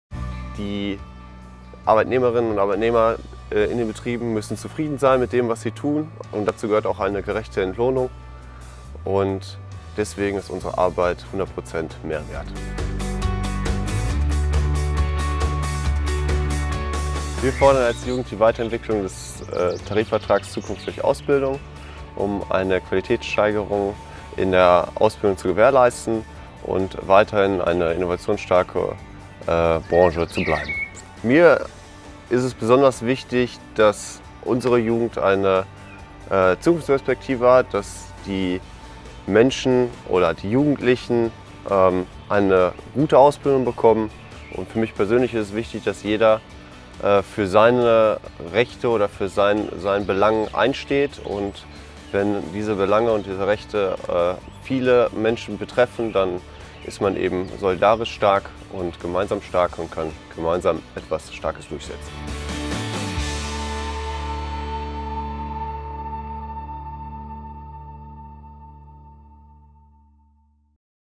Chemie2016 stellt sich vor: Sebastian Burdack
Die Tarifrunde Chemie2016 läuft an. Wir stellen Euch Menschen vor, die sich in den kommenden Monaten stellvertretend für alle Kolleginnen und Kollegen einsetzen, wie etwa Sebastian Burdack.